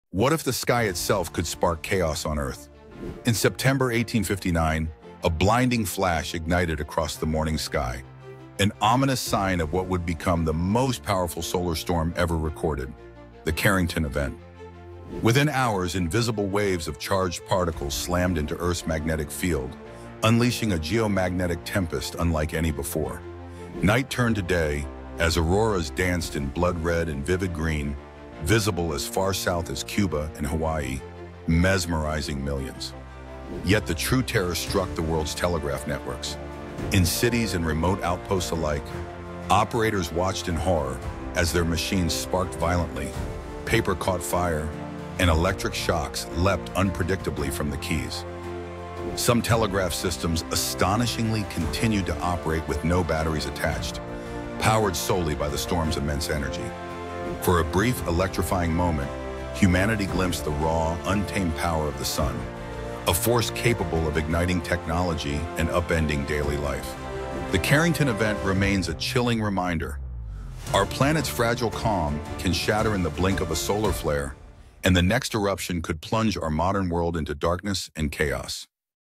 The Solar Blast That Threw Earth Into Chaos — And Could Happen Again
In 1859, Earth was hit by a solar blast so powerful it set telegraph systems on fire, lit up the skies like day, and pushed humanity to the brink—before we even understood what happened.

But here’s the terrifying part:
If the Carrington Event happened today, it could wipe out satellites, blackout continents, and crash the modern world in minutes.

Discover the solar storm that changed history—and why scientists warn we may not be ready for the next one.